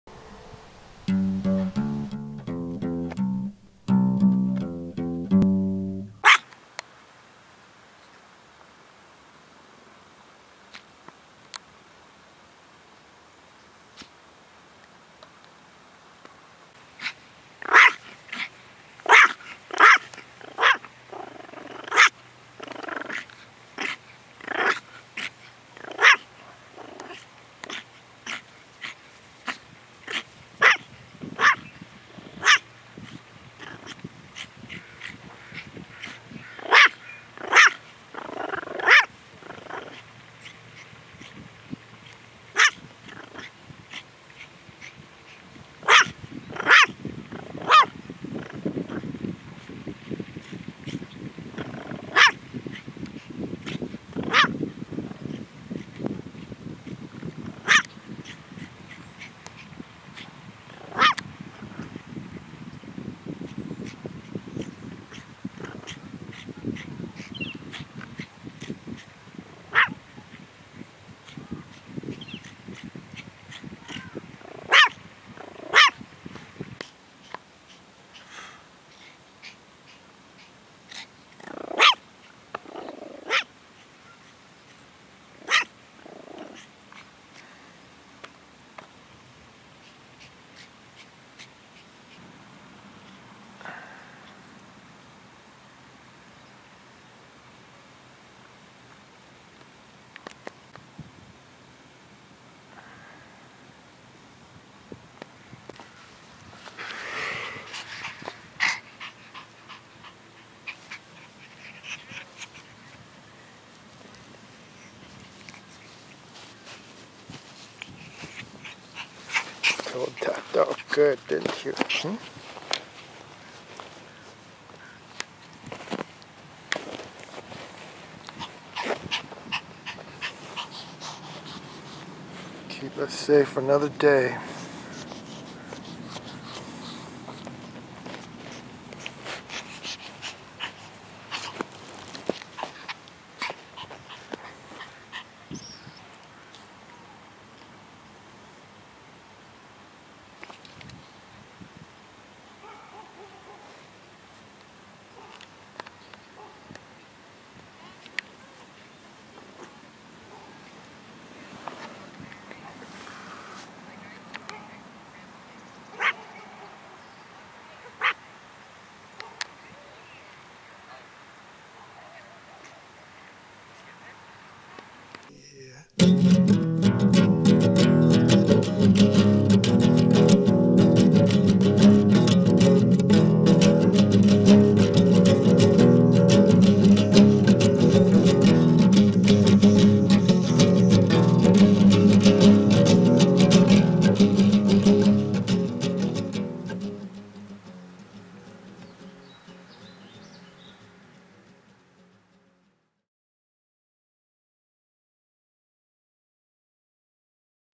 Guard Dog On Duty - EVE ep49
Pet doggy Eve, a long-haired toy Chihuahua in her natural habitat.  (documentary)
Supporting animal love and kindness.